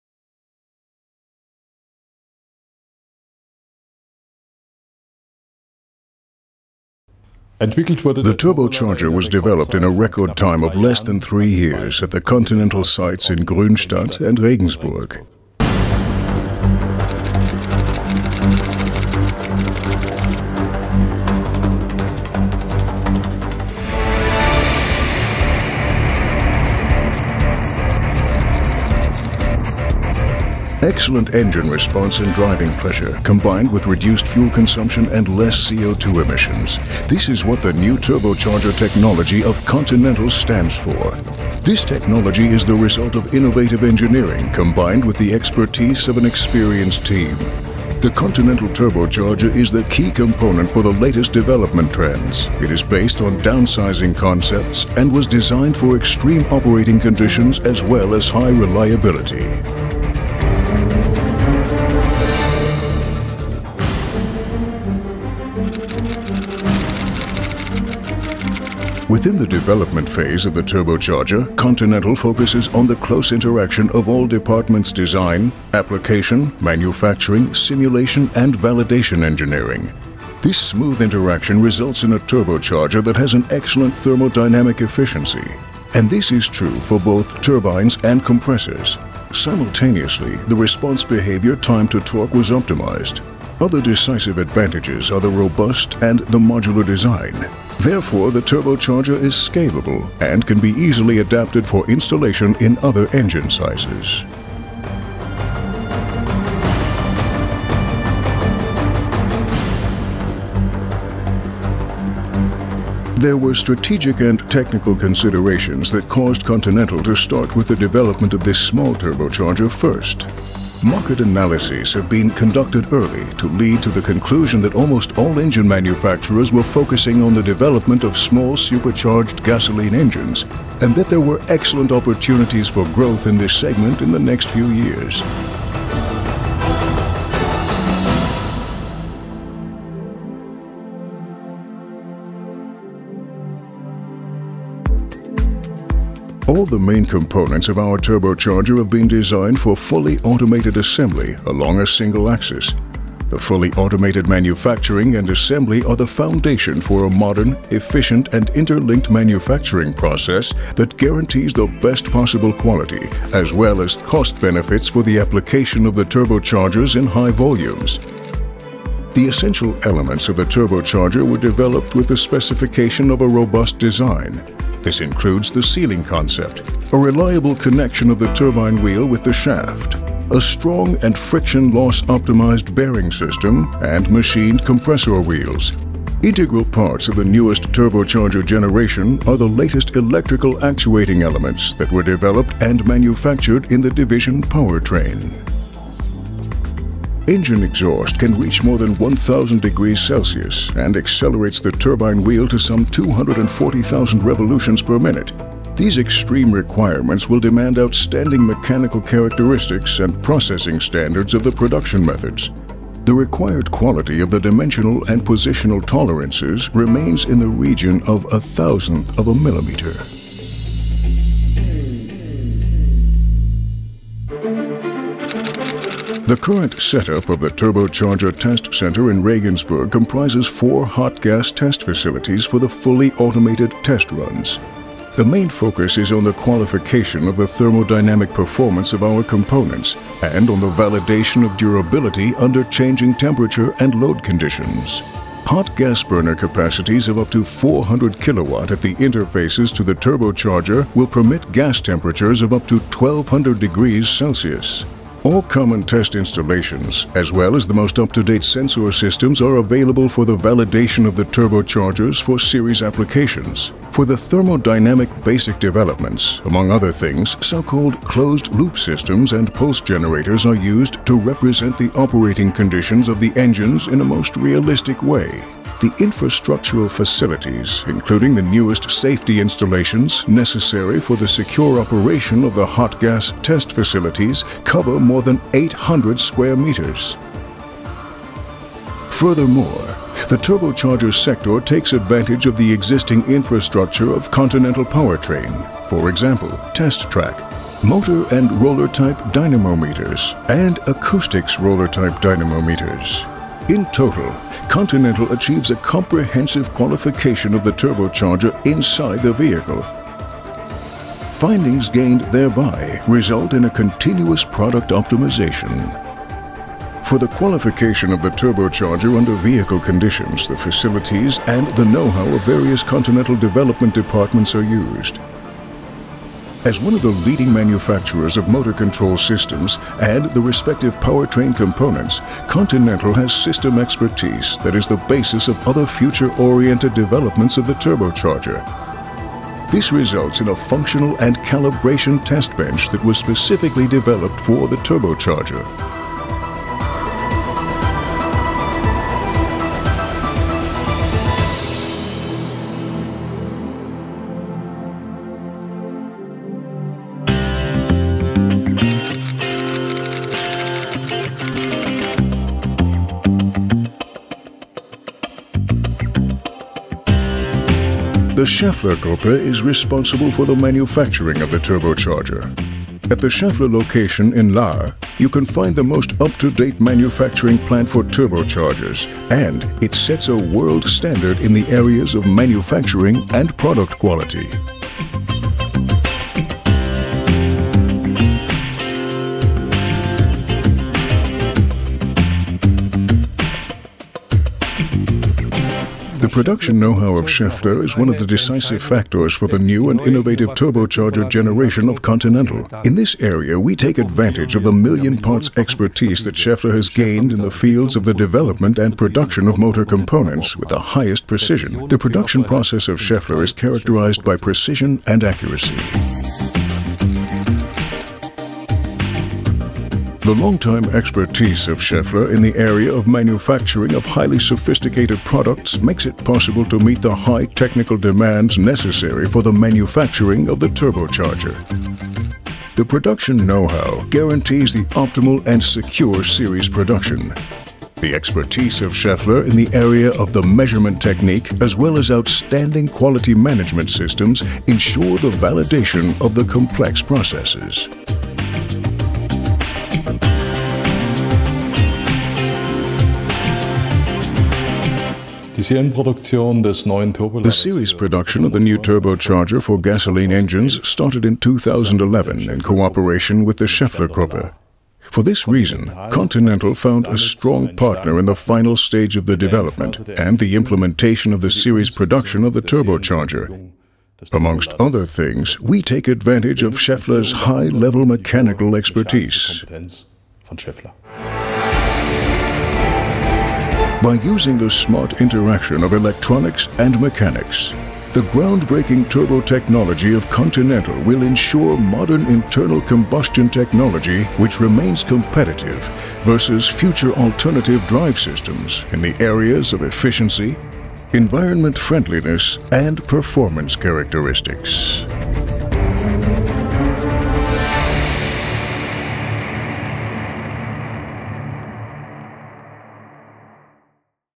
Continental - Imagefilm Turbolader
Imagefilm zur neuen Turbolader Generation der Continental Automotive GmbH und der Schaeffler AG